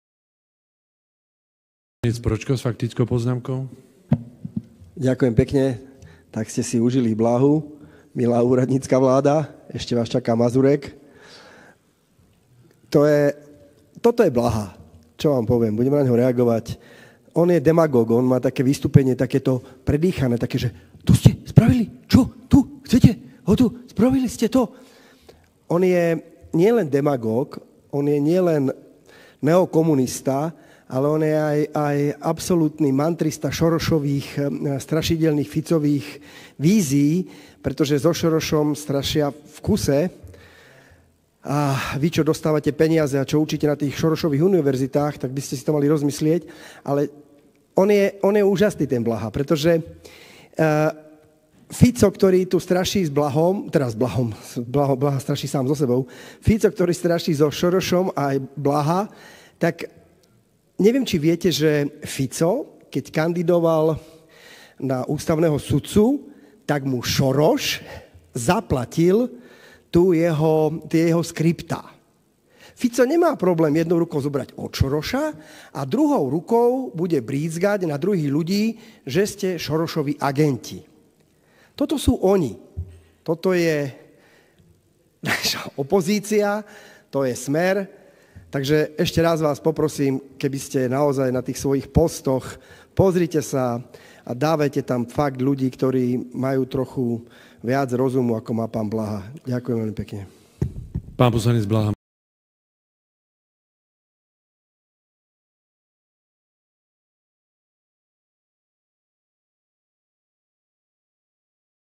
Jozef Pročko: Blaha je neokomunista
Jozef Pročko poslanec za klub Obyčajní ľudia a nezávislé osobnosti vystúpil v Národnej rade s faktickou poznámkou.